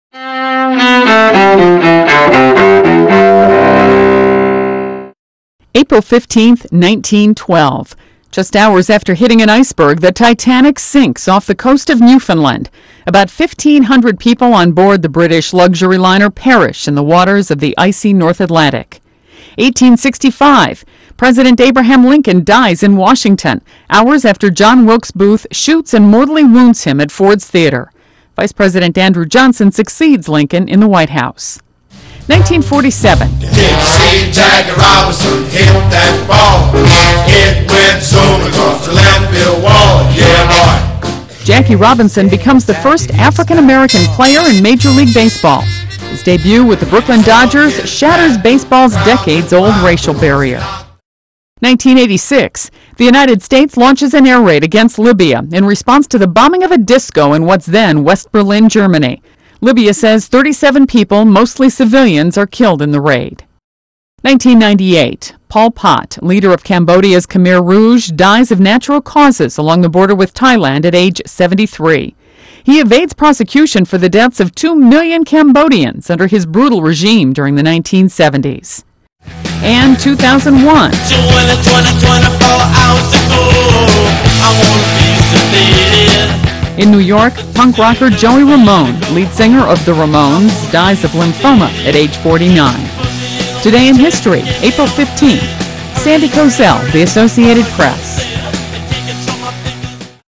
Today in History for April 15th
Highlights of Today in History for April 15th: Titanic sinks in Northern Atlantic; Abraham Lincoln dies; Jackie Robinson becomes first black player in Major League Baseball; US air raid on Libya; Pol Pot dies; Joey Ramone dies.